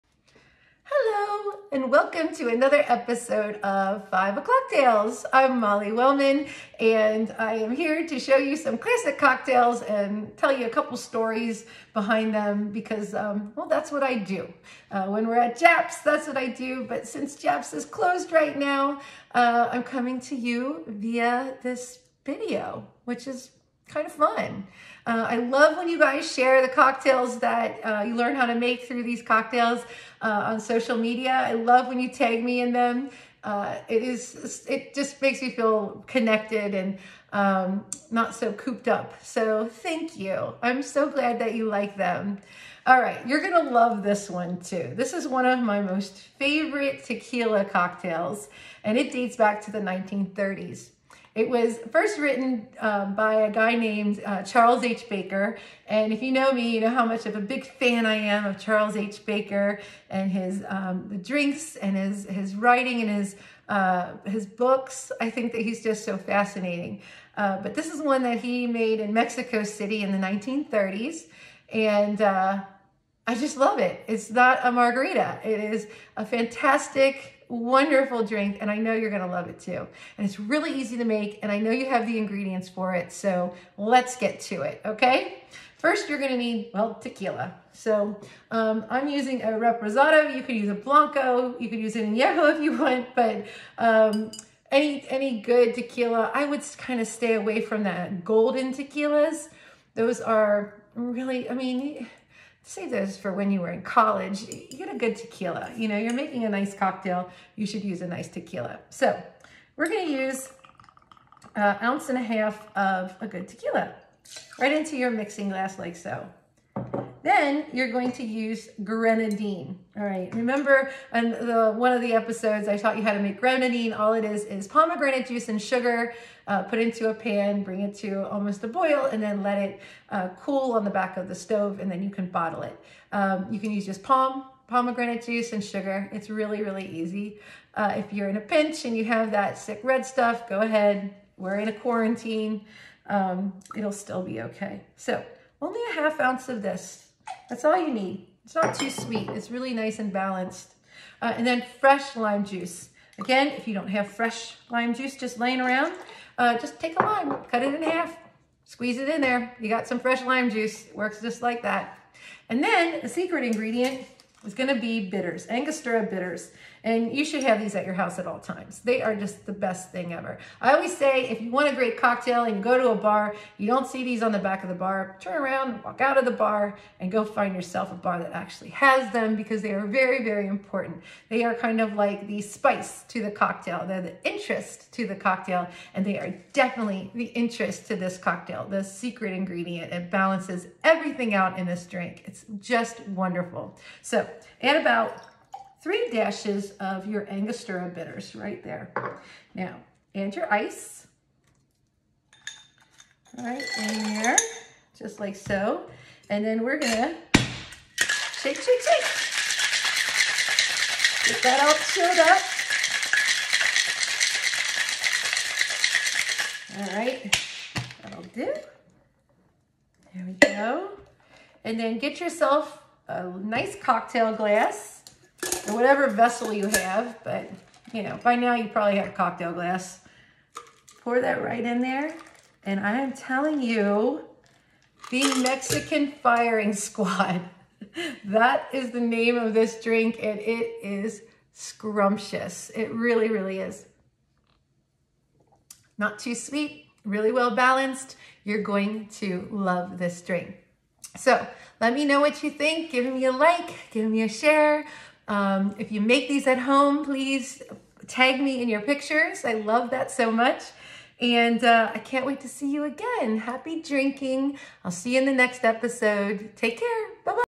Episode 12 - 5 O'Clock Tales with Molly Wellmann: Mexican Firing Squad
Join Japps' very own mixology maven, Molly Wellmann, as she details the history behind the Mexican Firing Squad cocktail and walks you through the step-by-step process of making your own at home!